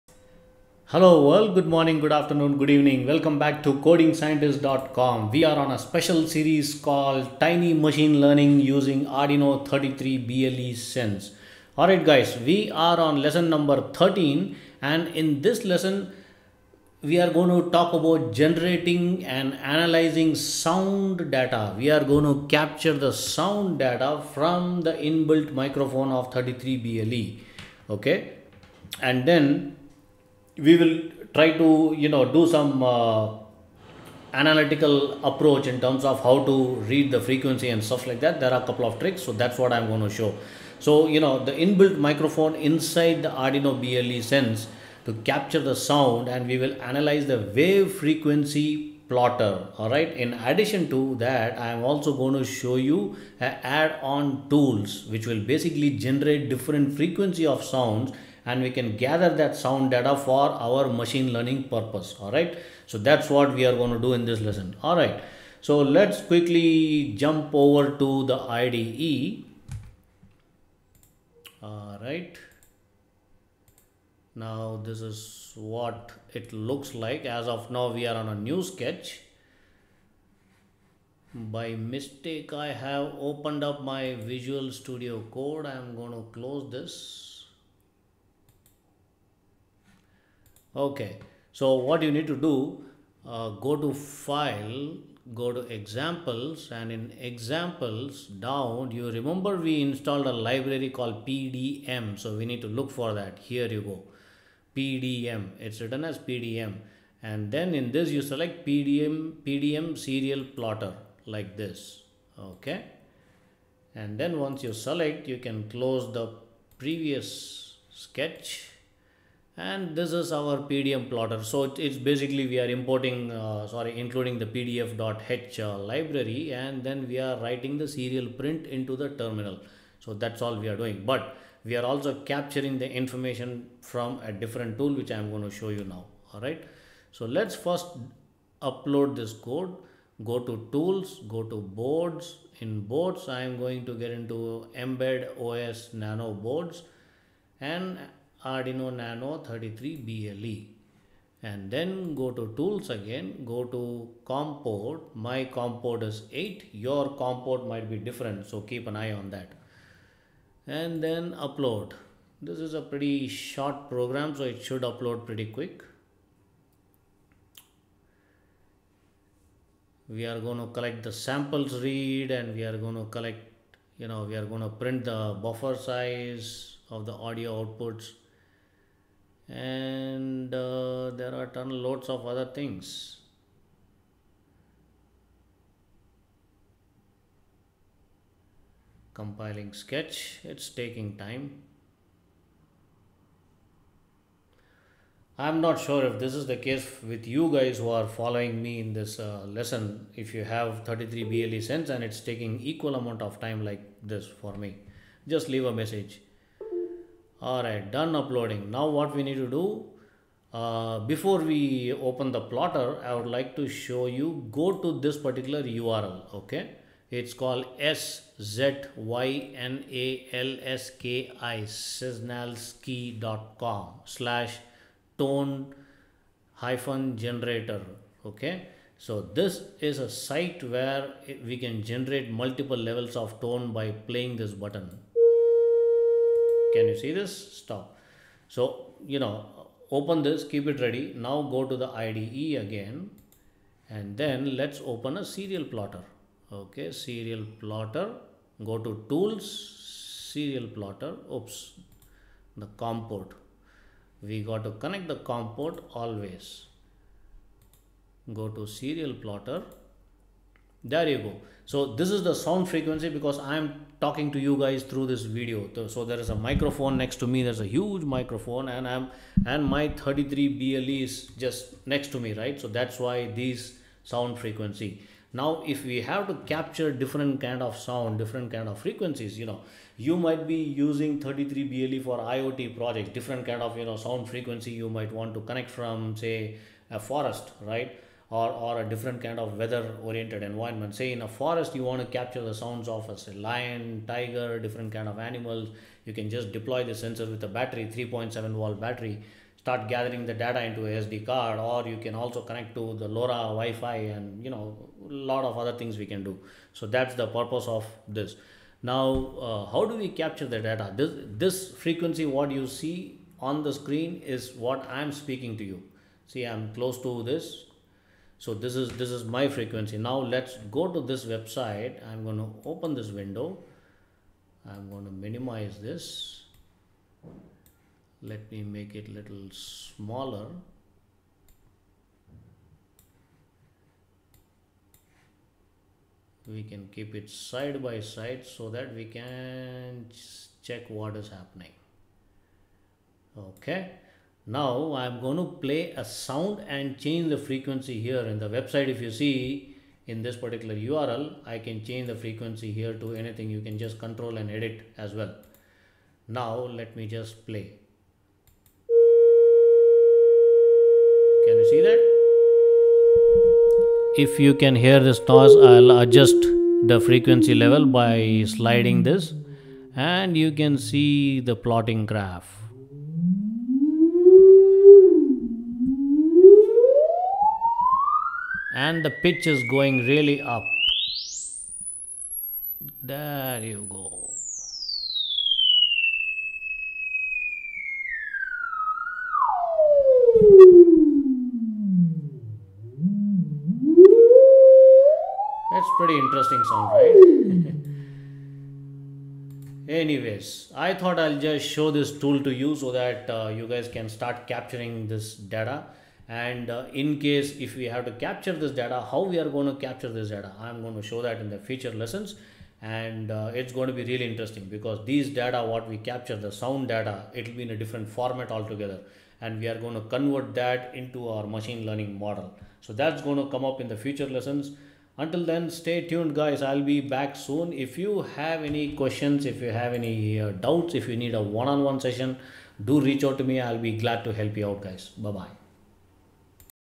Lesson 13 Generating and Analyzing Sound Data from Arduino BLE 33 Sense
In this lesson I will show you how to use the inbuilt Microphone inside Arduino 33 BLE Sense to capture sound and analyze the wave frequency plotter. In addition I will show you add on tools to generate sound to gather sound data for Machine Learning purpose.